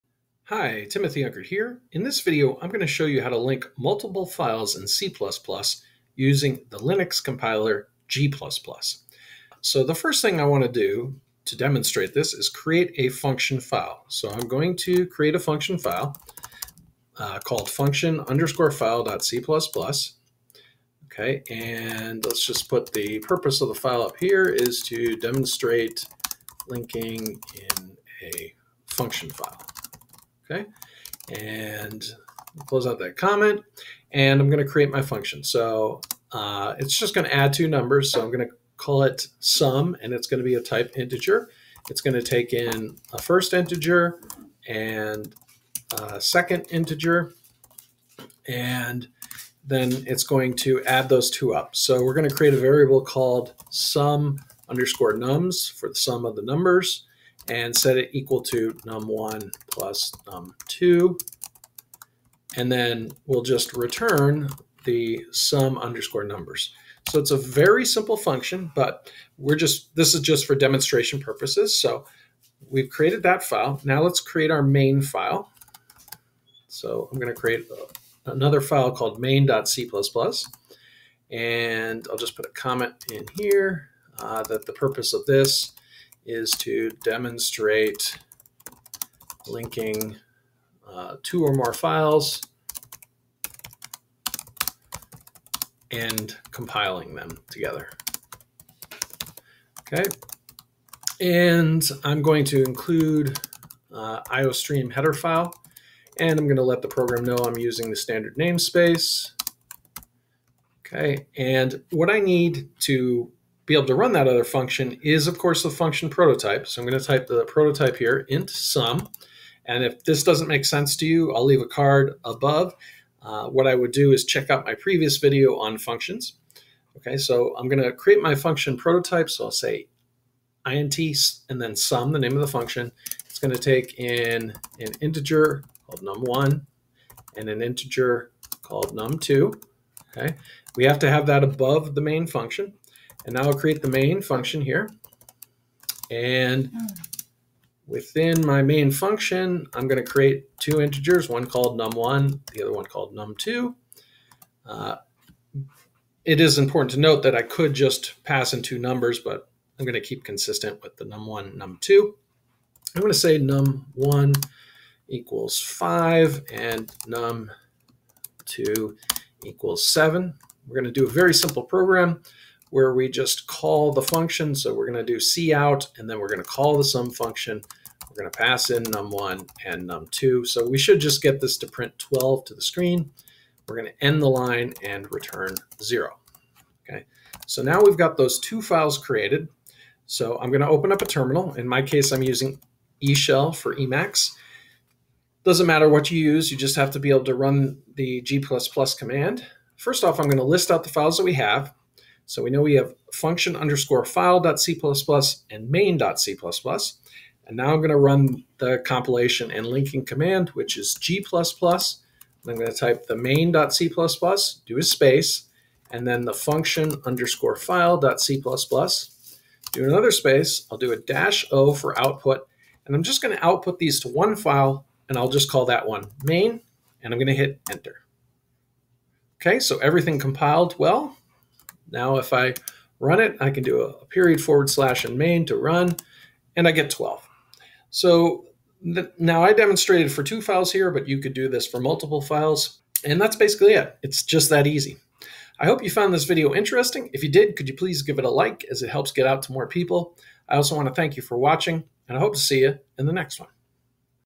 How to Link Multiple C++ Files with G++
In this C++ tutorial I show you how to compile and link multiple files in C++ with the g++ compiler available for Linux. I include a separate file for a simple function that returns a sum of two numbers and link and compile that to my main function in a separate file. The result is one file.

C++ logo attribution:
Jeremy Kratz, Public domain, via Wikimedia Commons

This file's color was altered from black to yellow and drop-shadow was added.